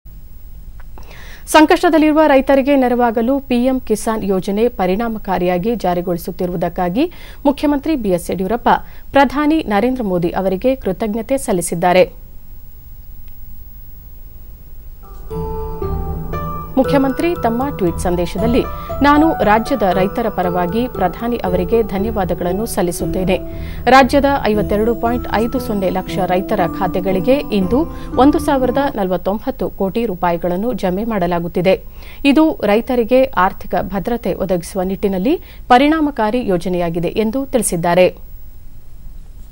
ಪಿಎಂಕಿಸಾನ್ ಪರಿಣಾಮಕಾರಿಯಾಗಿ ಜಾರಿಗೊಳಿಸುತ್ತಿರುವುದಕ್ಕಾಗಿ ಪ್ರಧಾನಿ ಅವರಿಗೆ ಕೃತಜ್ಞತೆ ಸಲ್ಲಿಸಿದ ಮುಖ್ಯಮಂತ್ರಿ
ಸಂಕಷ್ಟದಲ್ಲಿರುವ ರೈತರಿಗೆ ನೆರವಾಗಲು  ಪಿಎಂ ಕಿಸಾನ್  ಯೋಜನೆ ಪರಿಣಾಮಕಾರಿಯಾಗಿ ಜಾರಿಗೊಳಿಸುತ್ತಿರುವುದಕ್ಕಾಗಿ ಮುಖ್ಯಮಂತ್ರಿ ಬಿ.ಎಸ್. ಯಡಿಯೂರಪ್ಪ,  ಪ್ರಧಾನಿ  ನರೇಂದ್ರ ಮೋದಿ ಅವರಿಗೆ ಕೃತಜ್ಞತೆ  ಸಲ್ಲಿಸಿದ್ದಾರೆ.